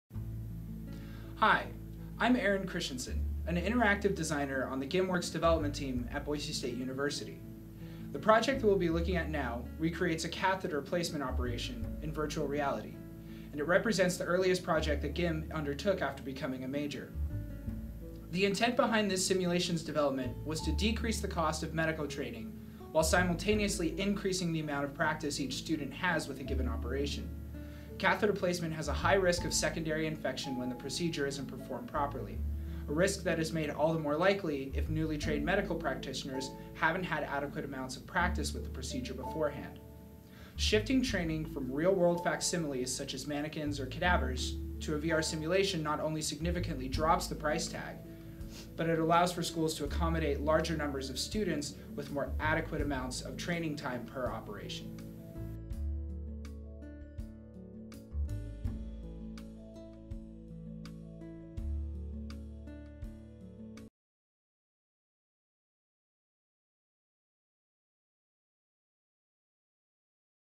VR Nursing Training Simulation
This project was created to teach nursing students how to correctly and safely perform a catheter insertion. It won the 2015 WOW award and is currently being used at universities across the world.